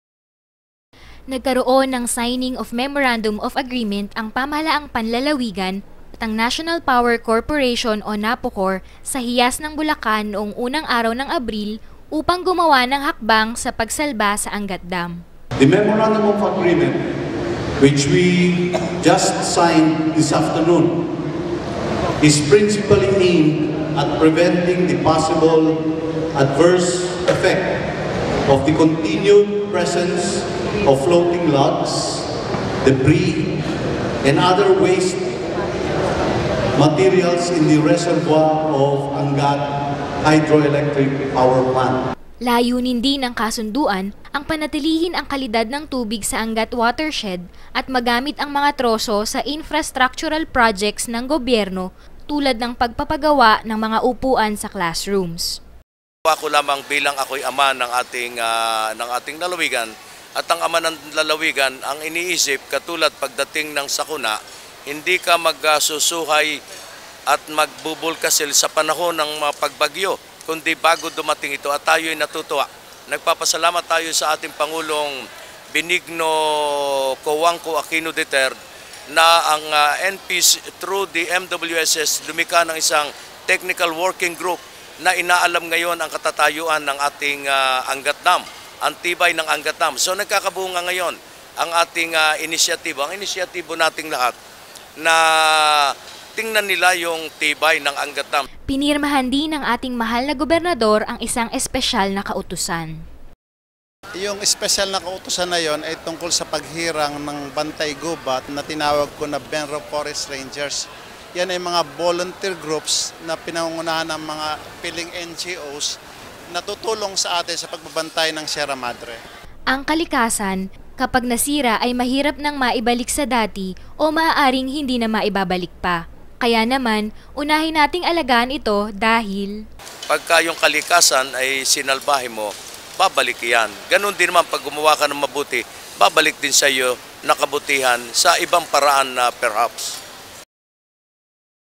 Galing Bulacan- MOA Signing between BENRO and NAPOCOR
Because of consecutive storms in Bulacan, Angat dam suffers crack damage. NAPOCOR and the Provincial Government of Bulacan signed a memorandum of agreement to help the said dam.—PPAO